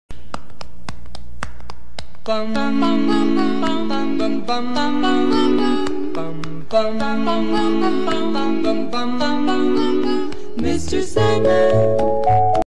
Tic tok. ALFEE CUTE LHASA APSO DOG
Tic tok video of ALFEE LHASA APSO DOG ENJOYING.